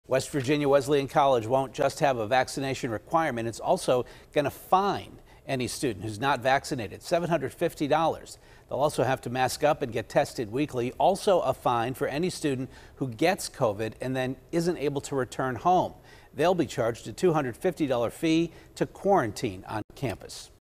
West Virginia Wesleyan To Fine Unvaccinated Students
A university in West Virginia is requiring vaccinations and will fine unvaccinated students $750.